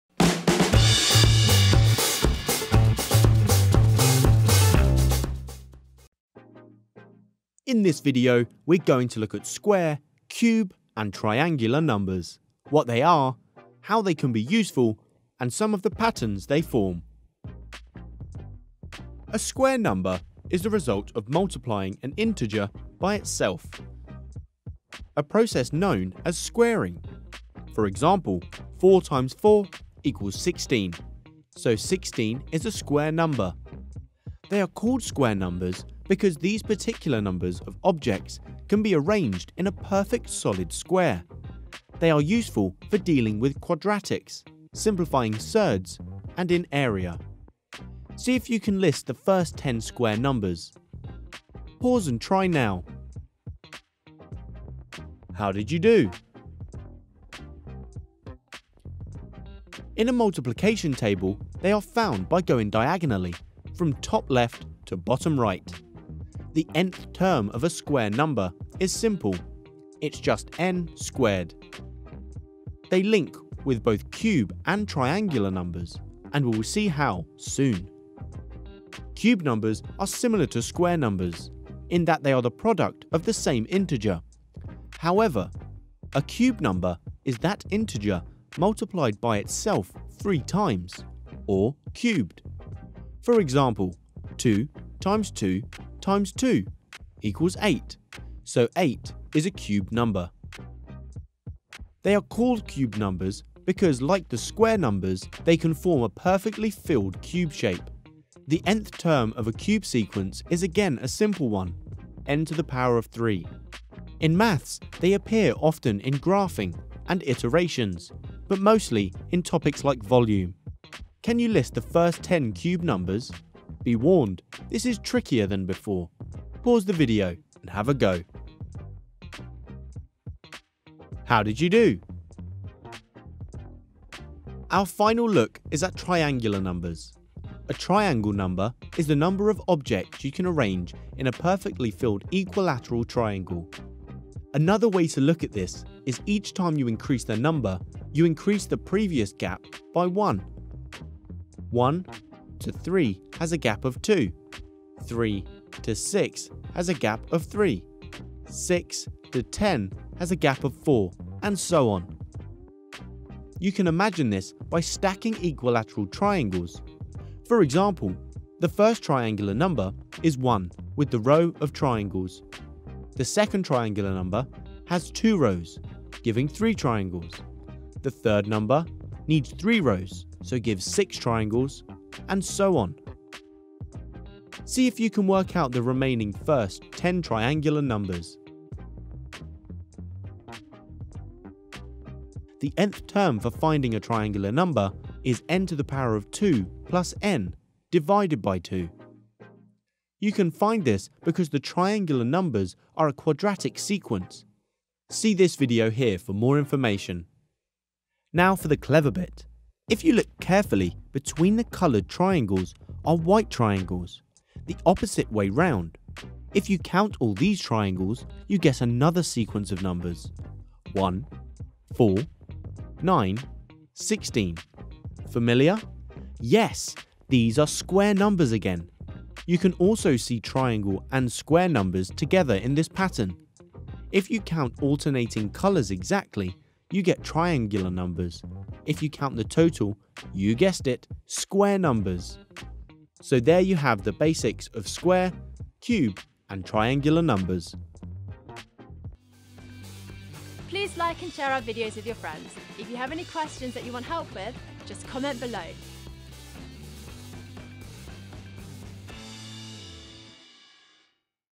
Square, Cube & Triangular Numbers | Maths | FuseSchool
In this video we are going to look at square, cube and triangular numbers, what they are, how they can be useful and some of the patterns they form

CREDITS
Animation & Design: Jean -Pierre Louw (Behance.net/Jean-Pierre_Louw)
Narration: Dale Bennett
Script: Joe Lewis